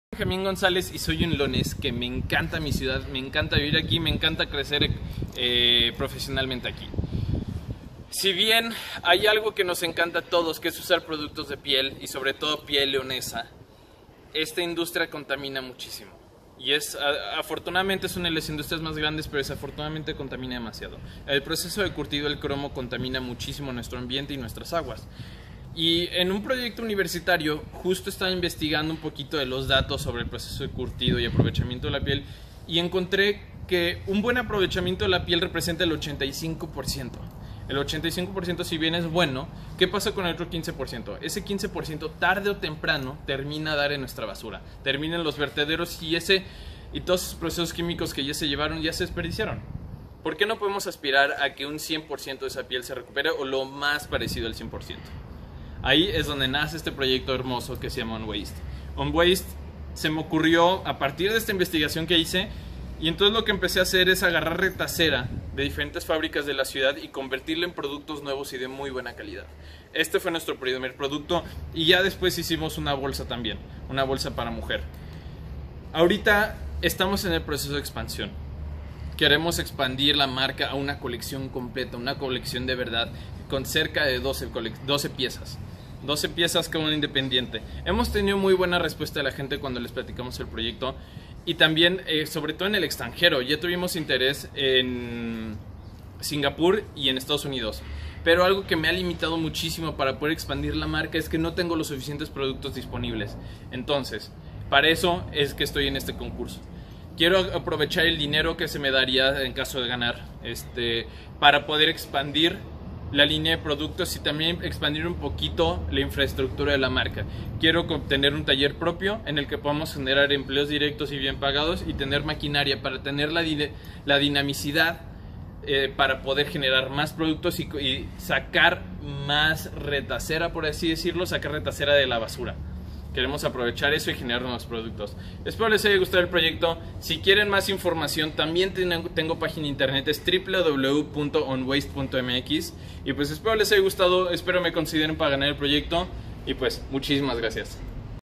Benjamin Gonzalez para el Premio Municipal Juventudes
La industria cuero/calzado es de las más importantes pero de las más contaminantes de la ciudad. UnWaste la cree para ayudar a reducir la cantidad de desperdicio de piel que termina en nuestros basureros.